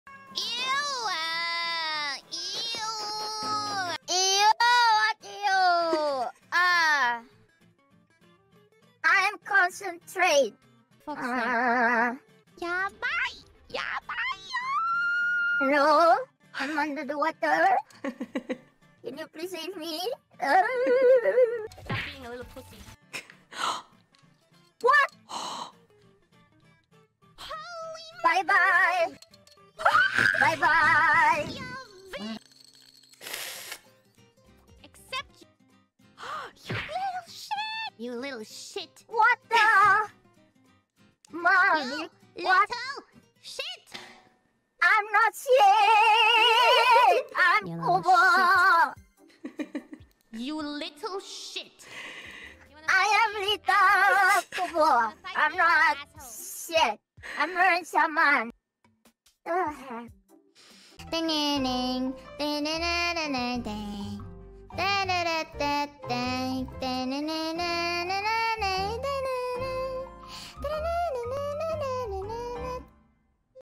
Kiara 媽媽輸了遊戲後生氣地罵孩子Kobo 是LITTLE SH*T? Kobo 突然嚇壞了？【Takanashi Kiara】【Kobo Kanaeru】【Hololive 中文精華】
精華來自：
【OPEN CALL IN】Play Boardgames With Sick Kiwawa & Soundboard!